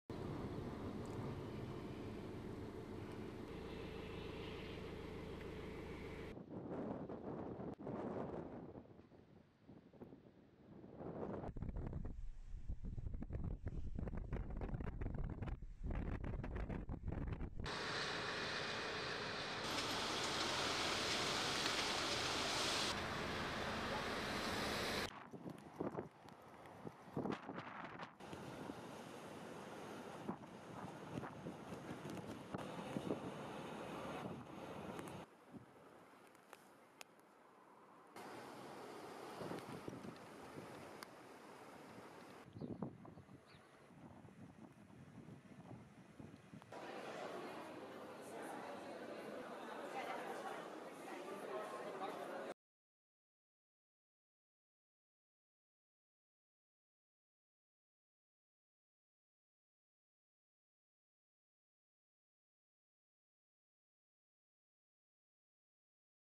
Windstorms Hit Many Parts of Greece
Windstorms hit many parts of Greece this week, causing traffic accidents and the suspension of flights and ferry services.

The wind was so strong that it was able to blow an unloaded truck off of an overpass in Corinth on Thursday, blocking traffic on the road.

The high winds brought surging sea weaves to Crete, Athens and Piraeus, disrupting local ferry services.

The severe weather also caused flight delays and suspensions at the airport in Crete, stranding many passengers.